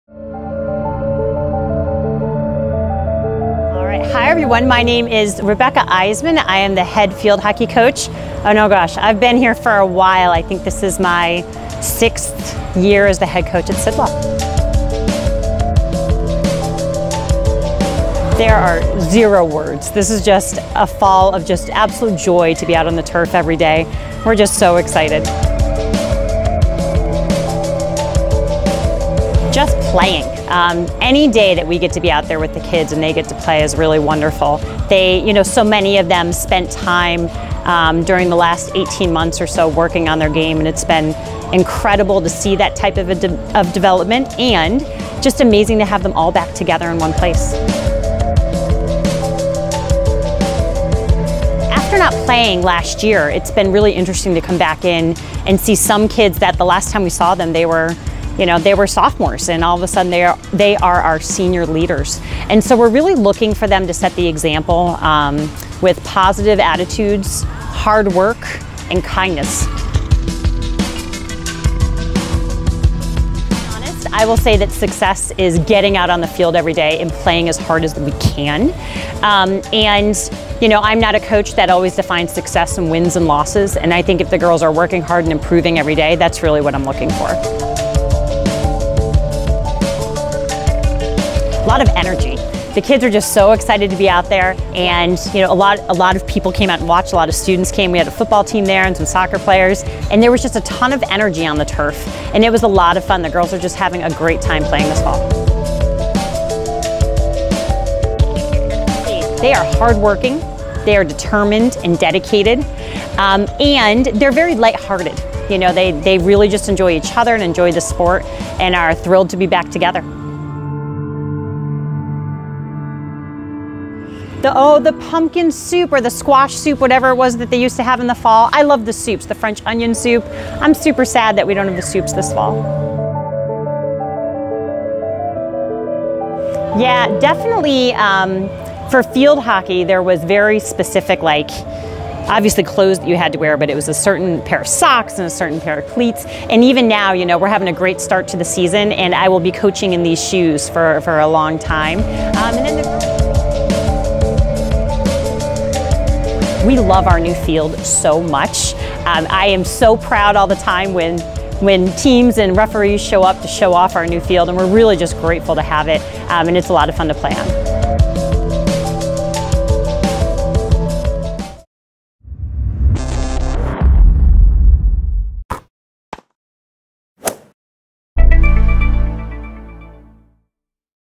Field Hockey Fall 2021 Outlook
Fall 2021Field Hockey season outlook with head coach Rebeckah Iseman.

Sidwell Friends Athletics.